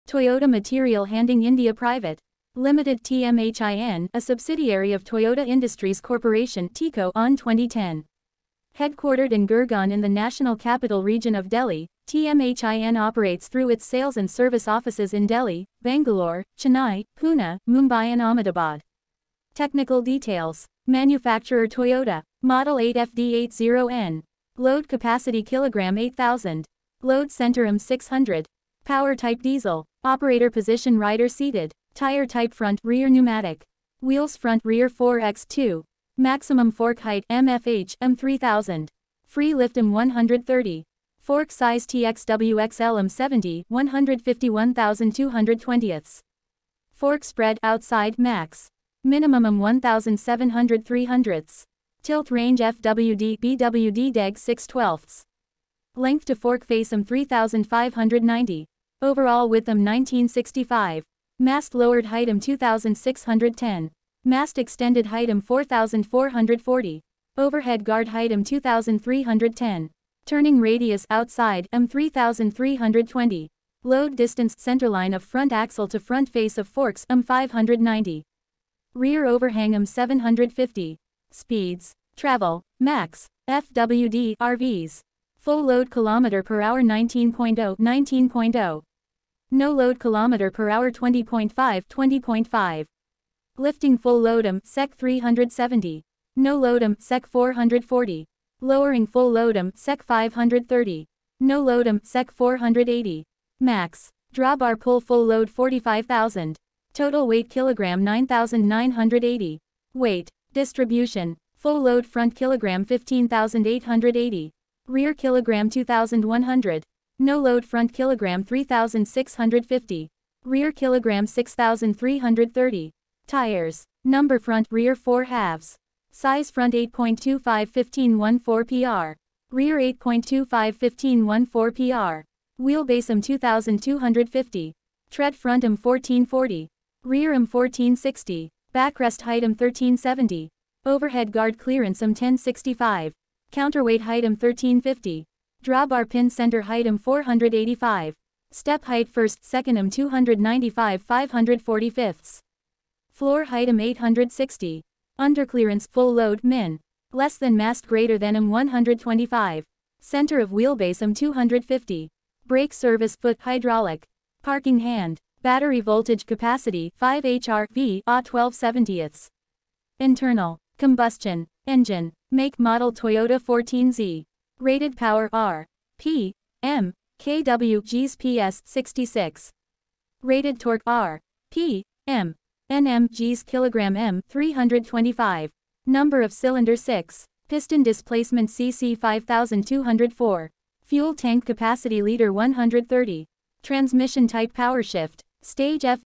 Toyota 8 Ton forklift vehicles technical details
Toyota 8 Ton forklift technical details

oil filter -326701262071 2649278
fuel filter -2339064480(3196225
air filter -P828889
air filter second -P829333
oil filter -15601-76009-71

transmission -ATF dexon
hydraulic -H-32
differential -GL-80w 90
grease -EP2